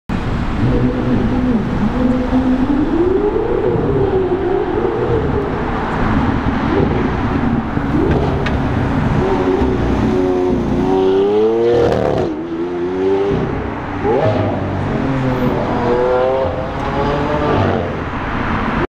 Old but gold! 😱
Obviously it's an Aventador LOL
All the best clips of Supercars on the streets.

Please do not use my clips in your videos without permission.